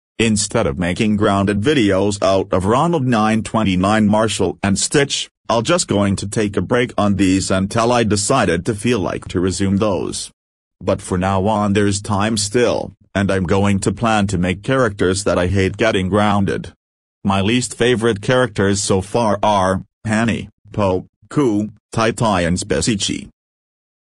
Annoucement march 29 2023
well gues what im gonna do so and that's why i'm taking a break for making grounded videos out of ronald929marshal and stitch

i'm going to focus on other characters that i hate or users at this time